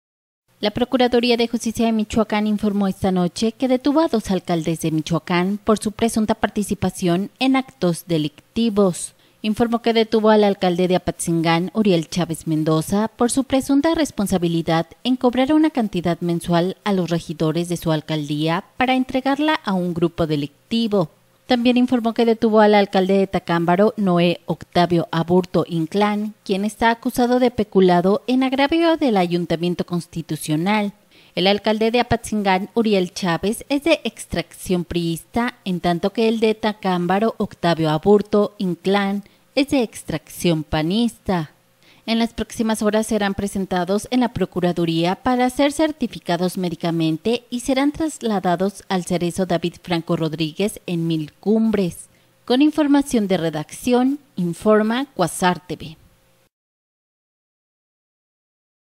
Detienen a alcaldes de Apatzingán y Tacámbaro
La procuraduría de justicia de Michoacán informó esta noche que detuvo a dos alcaldes de Michoacán por su presunta participación en actos delictivos...